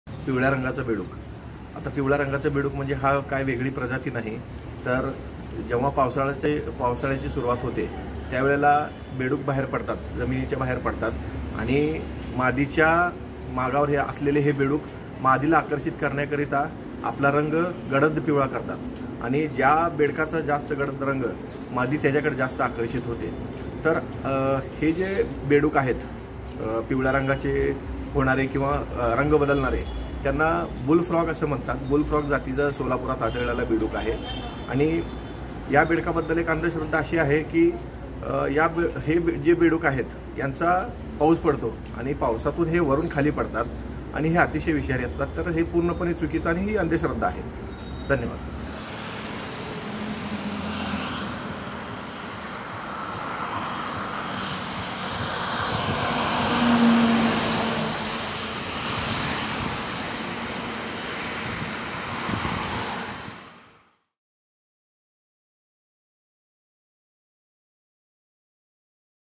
Yellow Frogs | पिवळा बेडूक आढळला | LetsUpp Marathi
सोलापूर येथे पिवळ्या रंगाचा बेडूक आढळून आला आहे. हा बेडूक जखमी झालेल्या अवस्थेत होता. त्याच्यावर उपचार करून त्याला निसर्गात सोडून देण्यात आले. हा बेडूक एक महिना पिवळा रंगाच्या अवस्थेत असतो. त्यानंतर पुन्हा मूळ रंगात येतो.